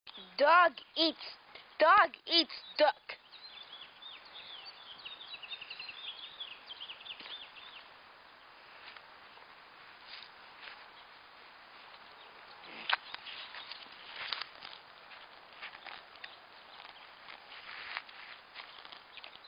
dog eats duck
a strange dog eats a duck